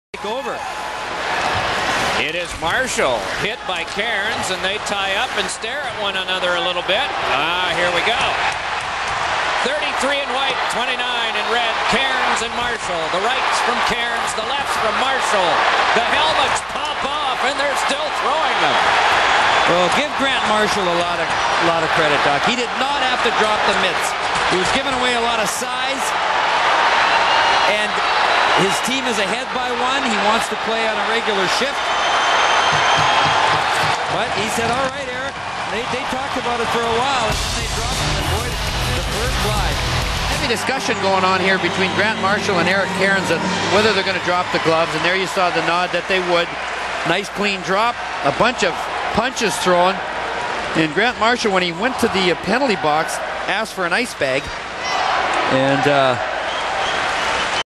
Cairns vs Marshall Mar 28, 2004
Eric Cairns vs Grant Marshall from the Islanders at Devils game on Mar 28, 2004.